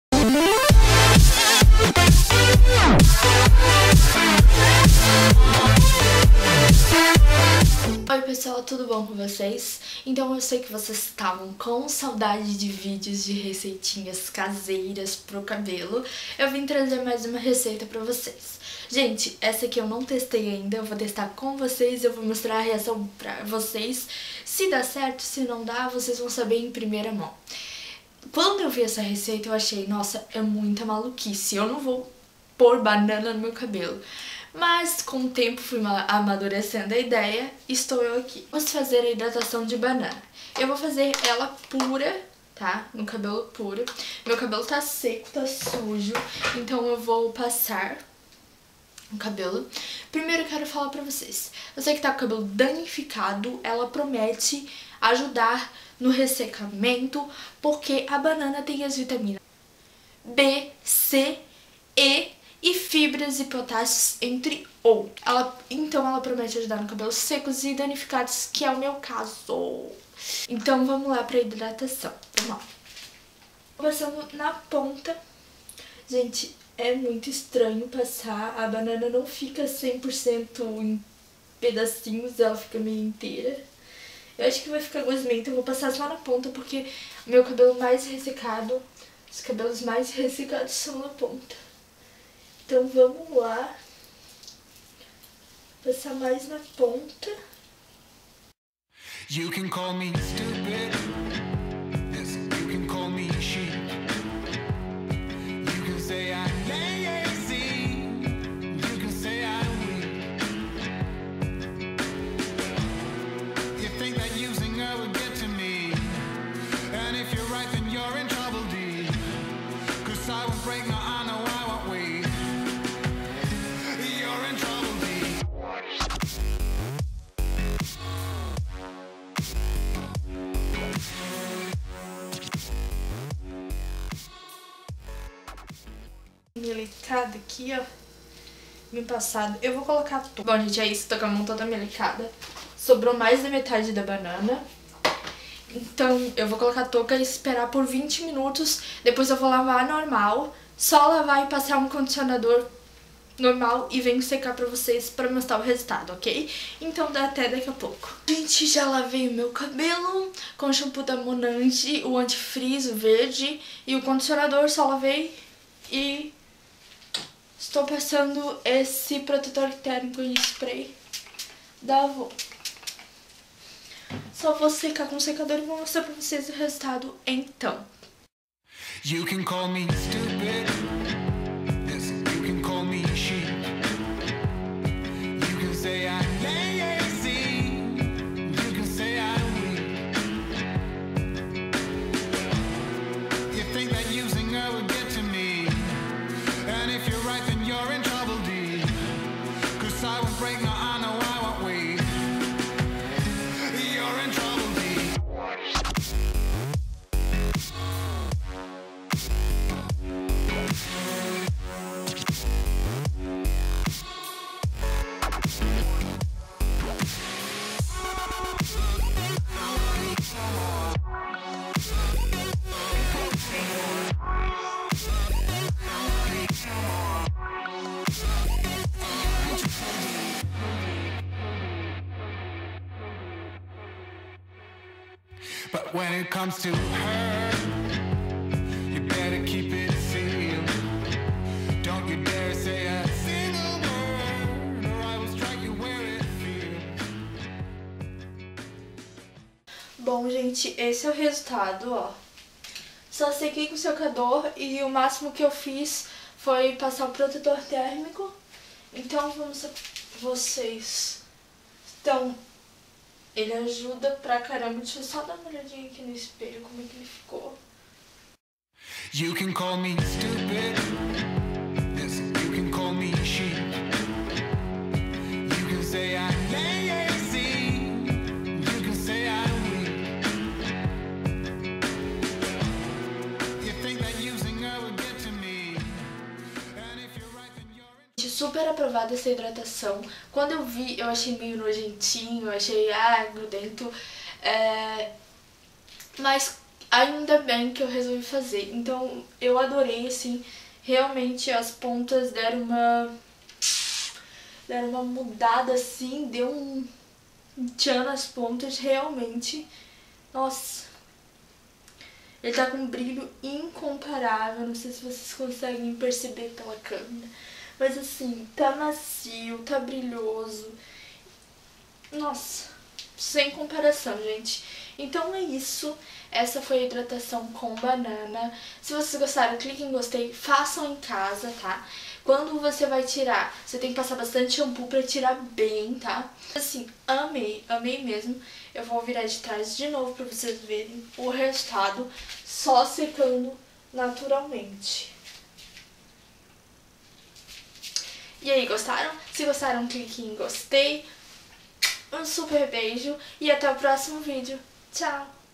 Mega Hidratação Com Banana
Oi pessoal hoje o video é sobre um experimento que resolvi testar ... sim com banana , Promete ajudar no ressecamento do cabelo , fortalecer as pontas e etc.. Quer saber o que eu achei? Vem conferir.. beijo